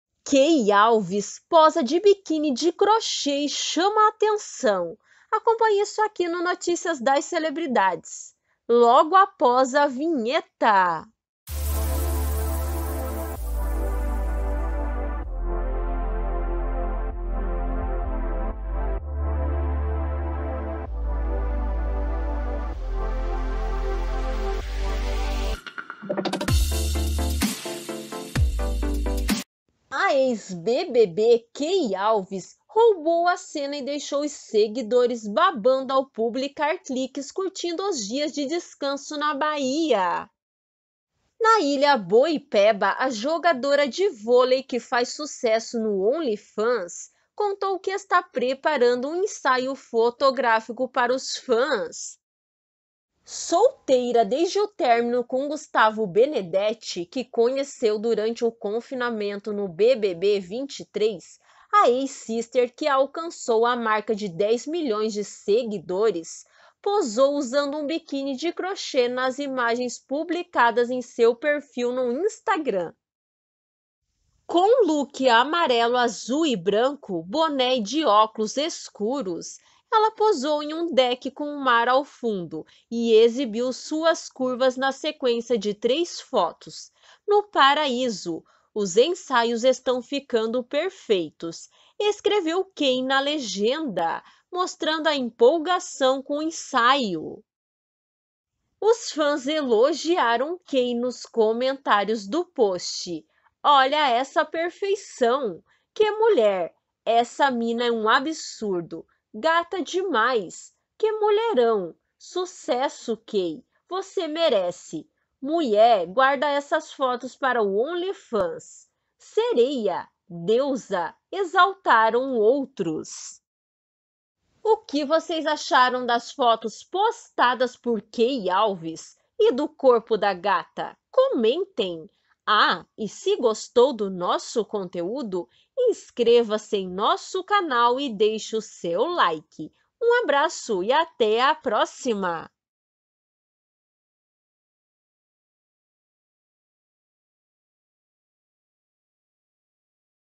KEY ALVES POSA DE BIQUÍNI DE CROCHÊ E CORPO CHAMA A ATENÇÃO!
Key Alves posa de biquíni de crochê e chama a atenção!

A ex-BBB Key Alves roubou a cena e deixou os seguidores babando ao publicar cliques curtindo os dias de descanso na Bahia!

Na Ilha Boipeba, a jogadora de vôlei, que faz sucesso no OnlyFans, contou que está preparando um ensaio fotográfico para os fãs.

Solteira desde o término com Gustavo Benedeti, que conheceu durante o confinamento no BBB 23, a ex-sister, que alcançou a marca de 10 milhões de seguidores, posou usando um biquíni de crochê nas imagens publicadas em seu perfil no Instagram.

Com o look amarelo, azul e branco, boné e de óculos escuros, ela posou em um deck com o mar ao fundo e exibiu suas curvas na sequência de três fotos. "No Paraíso. Os ensaios estão ficando perfeitos", escreveu Key na legenda, mostrando a empolgação com o ensaio.

Os fãs elogiaram Key nos comentários do post. "Olha essa perfeição. Que mulher", " Essa mina é um absurdo", "Gata demais", "Que mulherão", "Sucesso Key, você merece", "Muié, guarda essas fotos para o OnlyFans", "Sereia, deusa", exaltaram outros.

Assistam o vídeo até o final e comentem o que vocês acharam das fotos postadas por Key Alves! E do corpo da gata? Comentem!